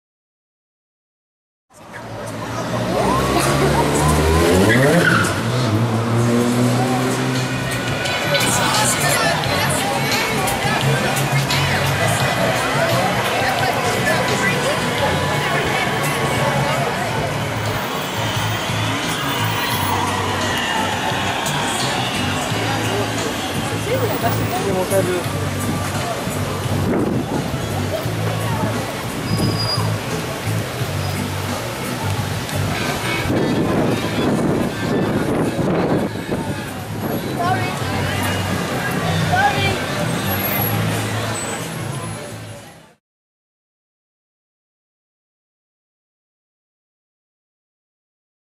THE COOLEST THING EVER??? Campagna T-Rex 3-wheeler
Jamboolio spots a two-seat, three-wheeled cyclecar called Campagna T-Rex.
It has a 1.4 L Kawasaki engine, cool LED lighting, carbon fiber parts in it and a massive stereo system. Gotta love this machine!

Specs:
Powered by a Kawasaki 1400 cc Inline 4 cylinder engine
197 horsepower
113 ft. lbs of torque
Incredible power to weight ratio: 5.29
Top speed: 144mph (230km/h)

Location: South Beach Miami, FL USA

Watch in High Definition (1080p HD).
Please rate (thumbs up!) & comment.